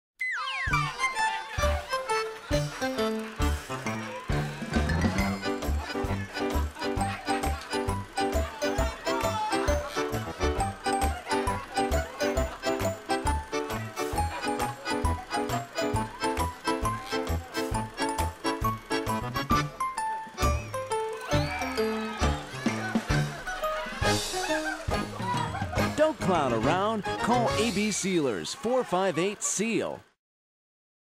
ABC LERS
Commercial for Branford Connecticut based paving company, ABC LERS. Produced by cameraman & editor Vic Roland in 2008. This commercial won a first place 2008 Telly Award. Vic also read the voice-over tag.